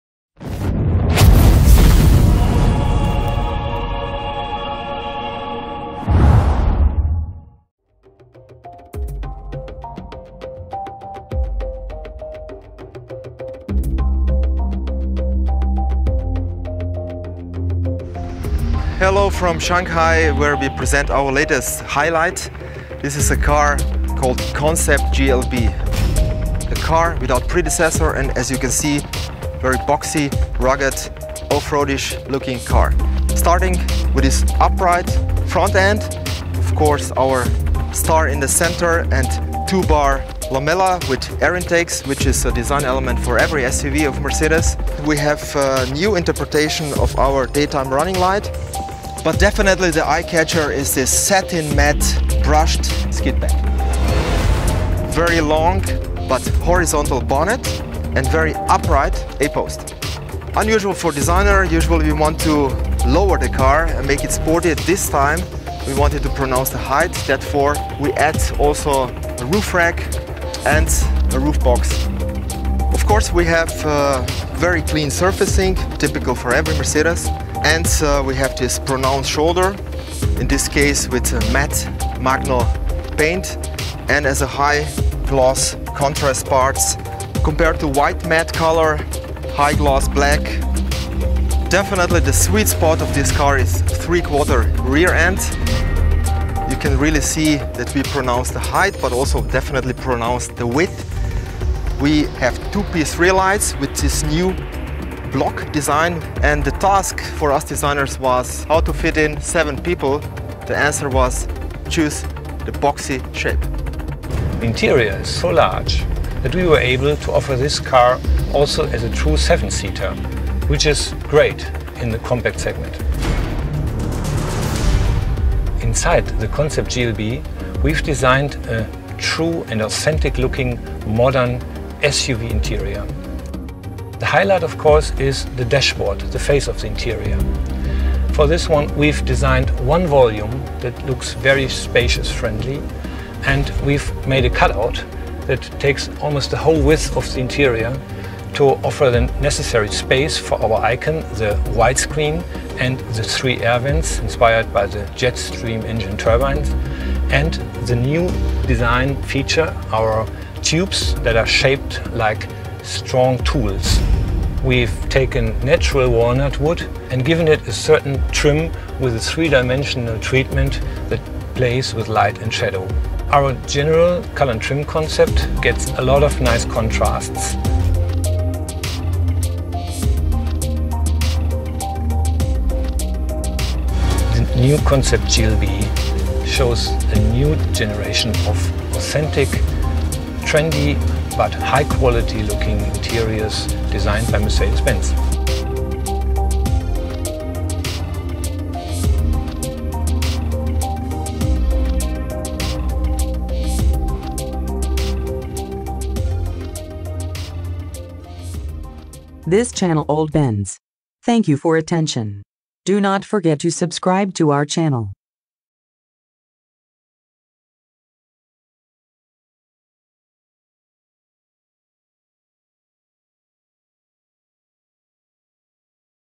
Mercedes-Benz Concept GLB - walkaround with Robert Lesnik and Hartmut Sinkwitz
The Mercedes-Benz Concept GLB has a distinctive presence in the world of compact SUVs with its muscular proportions. The clear, surface-accentuated contours with reduced lines and precise joints convey effortless superiority and modernity. The upright front section with its striking MULTIBEAM LED headlamps is clear evidence of off-road genes, as are the short overhangs at the front and rear. The long wheelbase and the functional greenhouse are the decisive factors providing generous spaciousness for up to seven occupants.

The muscular and sensuously contoured vehicle shoulder dominates the side view, an effect reinforced by the rising waistline. The contiguous doors improve ease of access and keep the door apertures free of soiling. All-around protective claddings divide the overall proportions and emphasize the vehicle's off-road character.

The harmonious color concept has exterior paintwork in design cashmere white Magno with high-gloss black mounted parts (e.g. the claddings or integrated roof box) as a contrast, plus discreet orange highlights in the radiator grille and as lettering on the outside mirror housings.

The chrome under guard at the front with integral air inlets in a stainless steel look emphasizes the off-road qualities. The rear under guard echoes this theme, creating a balanced overall picture.